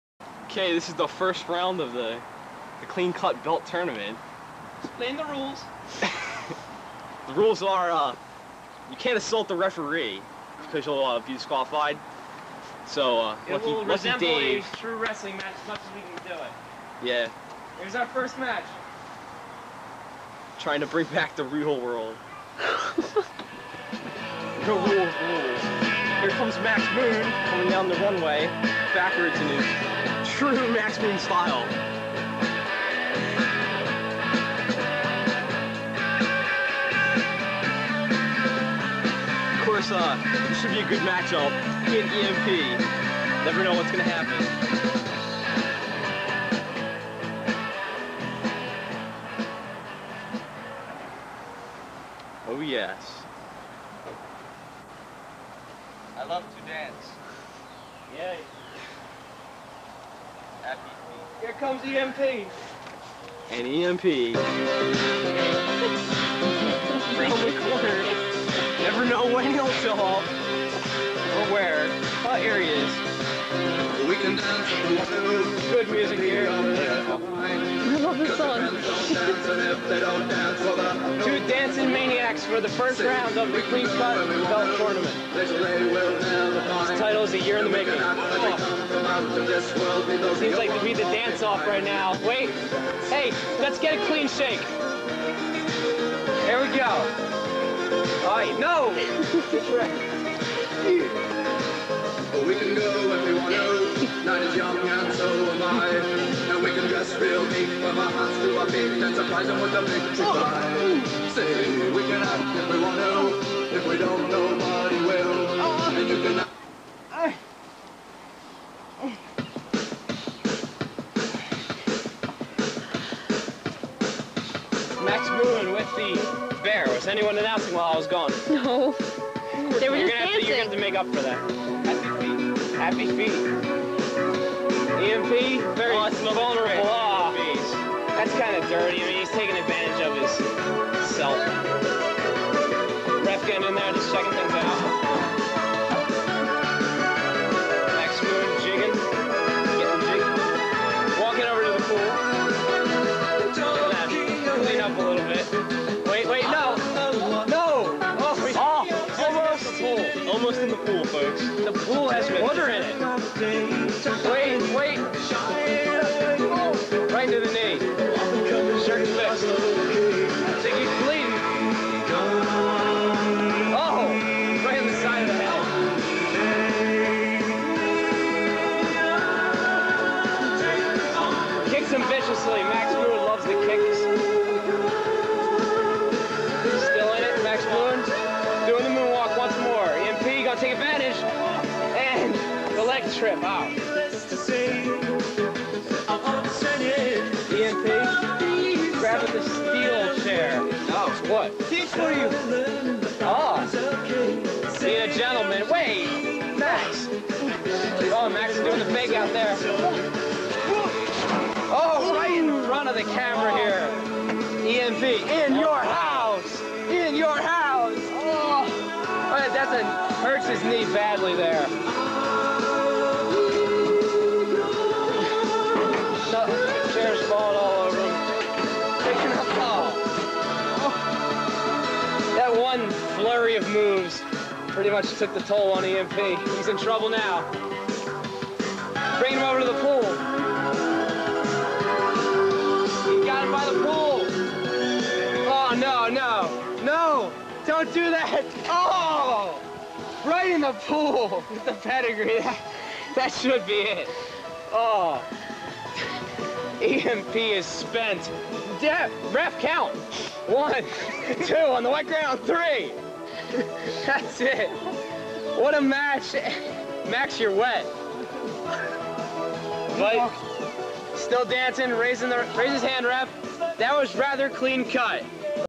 EMP vs. Max Moon
From Week 27 (6/18/99) which was the final week of classic BTW. Here Max Moon takes on the threat of EMP, also known as the Escaped Mental Patient. if you listen to the commentary you'll hear that this is a first round match in the Clean Cut Belt tournament. We were planning on having the tournament finals at the next BTW, but since the next BTW ended up being 9 years later it didn't end up happening. Max Moon was going to go over, too!

Aristocrab is next!